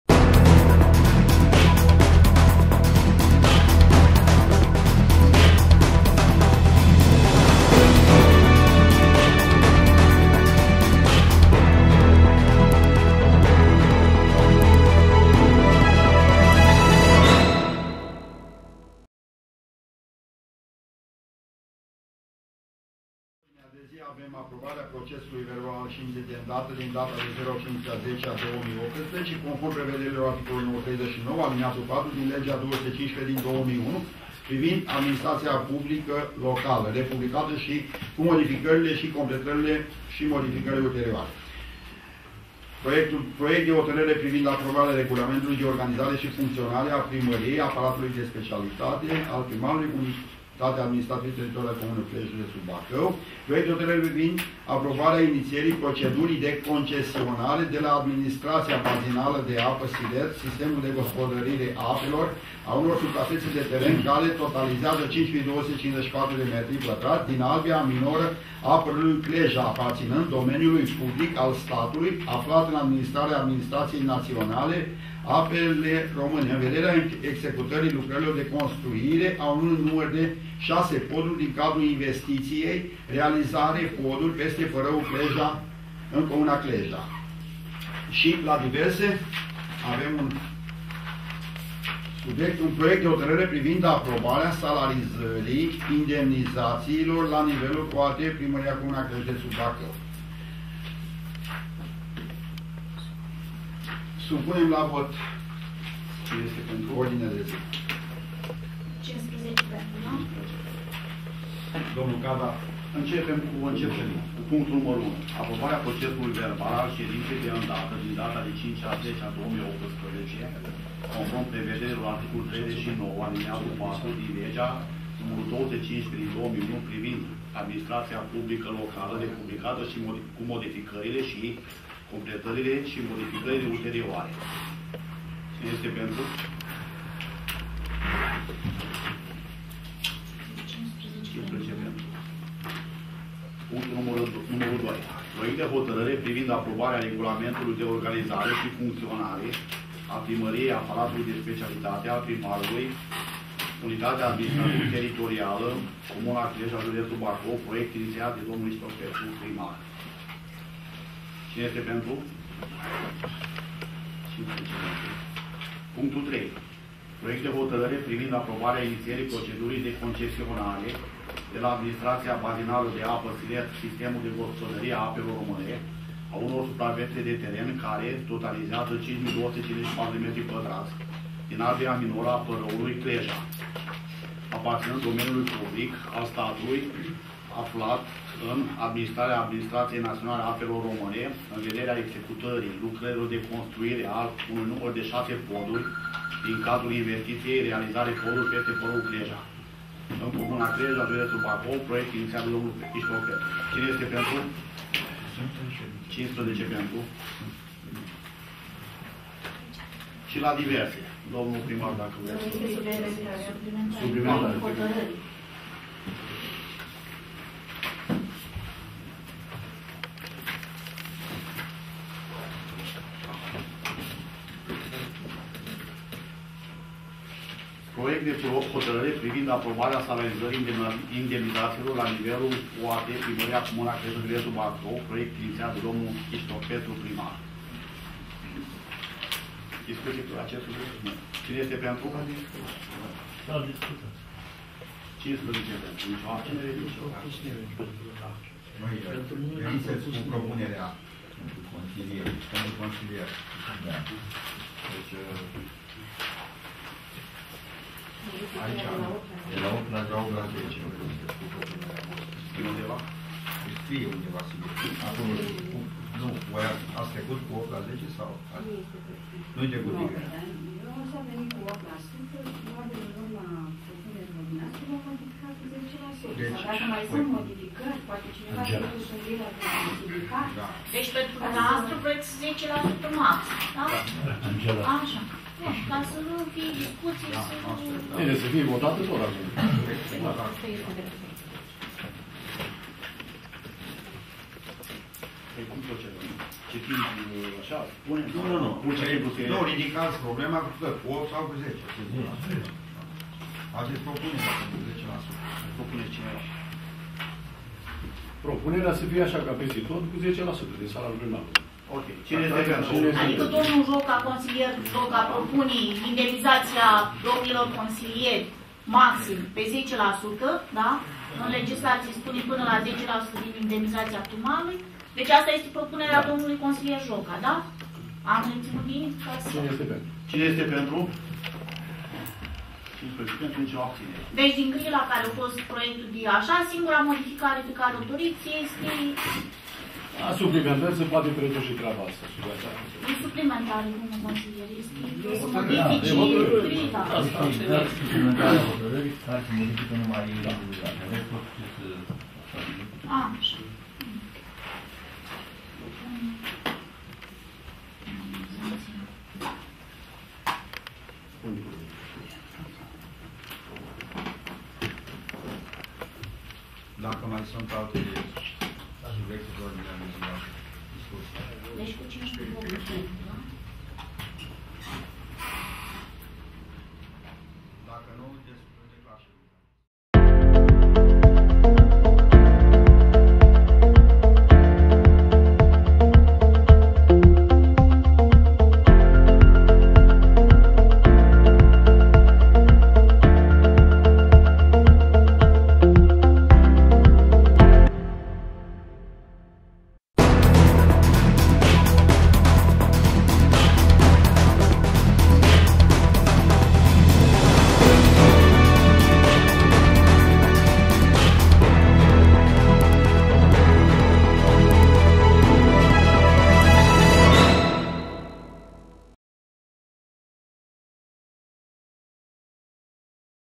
LA CLEJA Sedinta Consiliului Local din 16 oct FILMARE EXCLUSIV TV UHD 4K
LA CLEJA, județul Bacău, Ședința Consiliului Local din 16 octombrie 2018 FILMARE EXCLUSIV TV UHD 4K.

Cu stimă,
echipa Exclusiv

Vizionare plăcută !